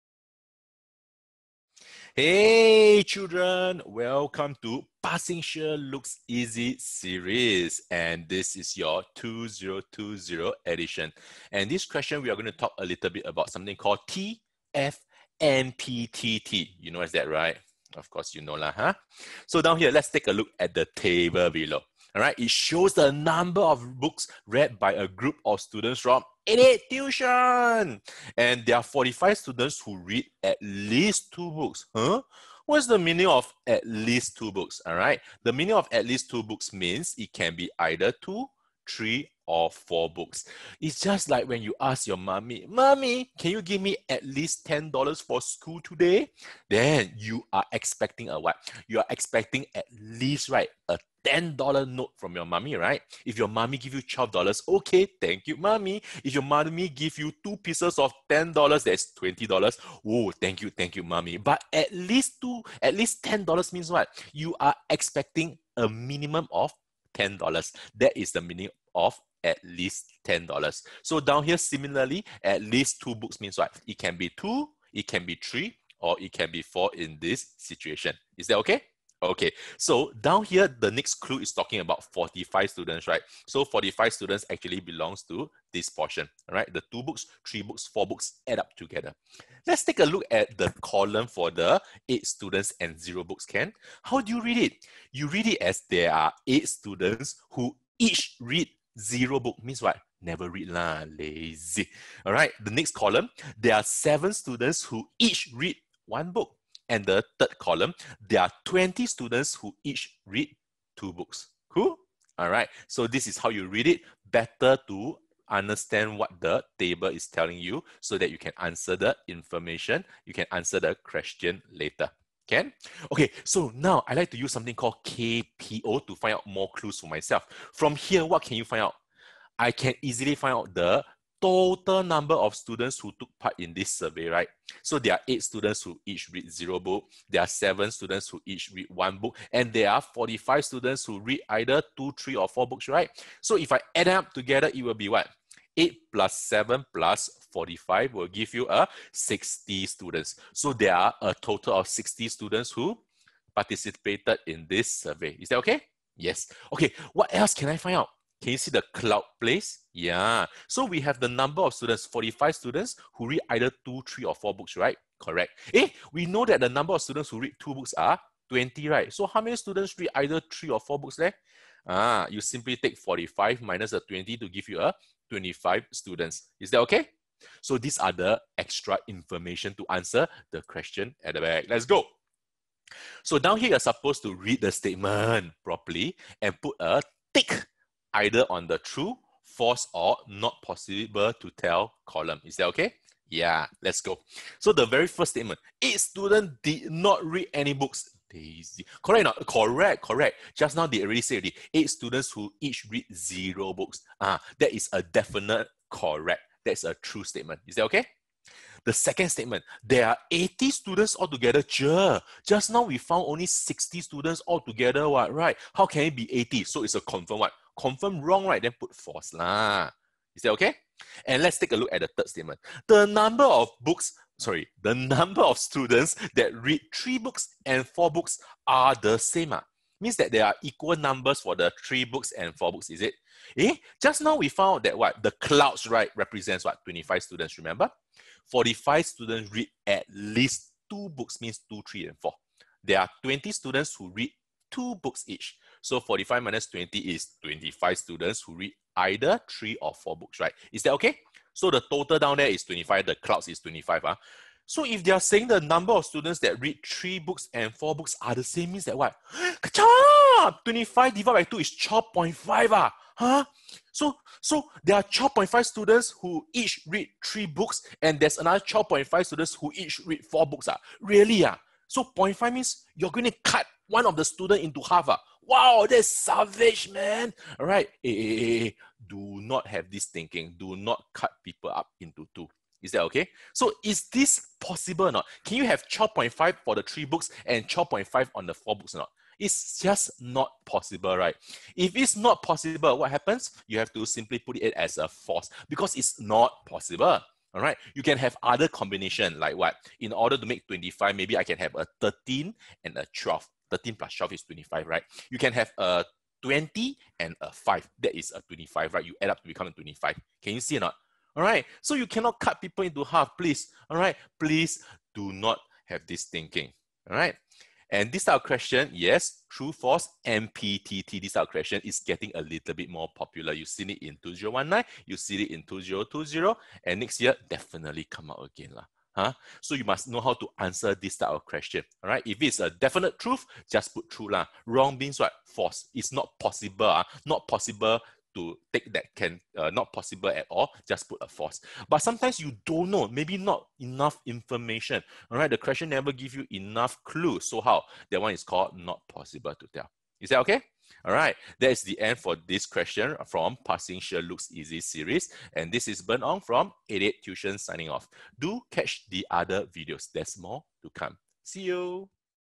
PSLE MATH 2020 - True / False / NPTT Questions | 88tuition
Get a clear understanding on how to answer True / False / Not Possible to Tell questions with proper reasoning. Learn T/F/NPTT questions with practical examples.